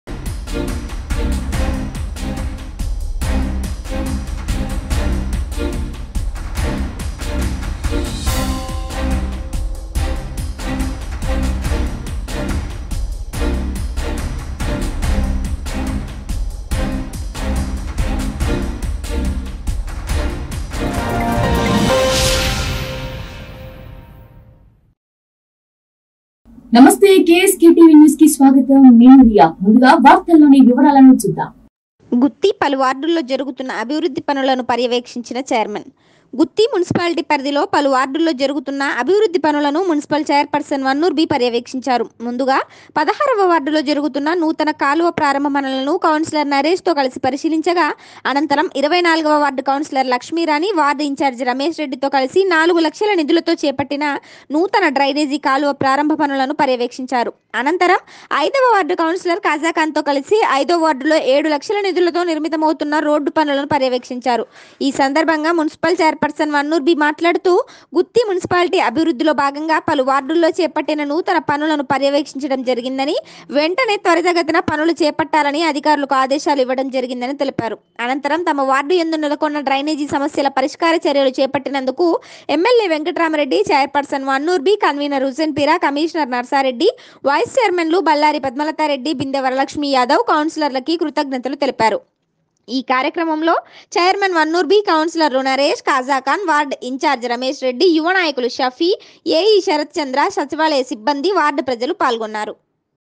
గుత్తి పలు వార్డుల్లో జరుగుతున్న అభివృద్ధి పనులను పర్యవేక్షించిన చైర్మన్.
గుత్తి మున్సిపాలిటీ పరిధిలో పలు వార్డుల్లో జరుగుతున్న అభివృద్ధి పనులను మున్సిపల్ చైర్ పర్సన్ వన్నూర్ బి పర్యవేక్షించారు.ముందుగా 16 వ వార్డులో జరుగుతున్న నూతన కాలువ ప్రారంభ పనులను కౌన్సిలర్ నరేష్ తో కలిసి పరిశీలించగా,అనంతరం 24 వ వార్డు కౌన్సిలర్ లక్ష్మీ రాణి వార్డ్ ఇంచార్జ్ రమేష్ రెడ్డి తో కలిసి నాలుగు లక్షల నిధులతో  చేపట్టిన నూతన డ్రైనేజీ కాలువ ప్రారంభ పనులను పర్యవేక్షించారు.అనంతరం ఐదవ వార్డు కౌన్సిలర్ కాజా ఖాన్ తో కలిసి ఐదో వార్డ్ లో ఏడు లక్షల నిధులతో నిర్మితమవుతున్న రోడ్డు పనులను పర్యవేక్షించారు.ఈ సందర్భంగా మున్సిపల్ చైర్పర్సన్ వన్నూర్ బి మాట్లాడుతూ గుత్తి మున్సిపాలిటీ అభివృద్ధిలో భాగంగా పలు వార్డుల్లో చేపట్టిన నూతన పనులను పర్యవేక్షించడం జరిగిందని,వెంటనే త్వరితగతిన పనులు చేపట్టాలని అధికారులకు ఆదేశాలు ఇవ్వడం జరిగిందని తెలిపారు. అనంతరం తమ వార్డు యందు నెలకొన్న డ్రైనేజీ సమస్యల పరిష్కార చర్యలు చేపట్టినందుకు ఎమ్మెల్యే  వెంకట్రామి రెడ్డి,చైర్ పర్సన్ వన్నూర్ బి,కన్వినర్ హుసేన్ పీరా, కమిషనర్ నర్సారెడ్డి,వైస్ చైర్మన్లు బళ్లారి పద్మలత రెడ్డి,బిందె వరలక్ష్మి యాదవ్,కౌన్సిలర్లు కి కృతజ్ఞతలు తెలిపారు.ఈ కార్యక్రమంలో చైర్మన్ వన్నూర్ బి, కౌన్సిలర్లు నరేష్,ఖాజా ఖాన్,వార్డు ఇంచార్జ్ రమేష్ రెడ్డి,యువ నాయకులు షఫీ,ఏ.ఈ శరత్ చంద్ర,సచివాలయ సిబ్బంది,వార్డు ప్రజలు పాల్గొన్నారు.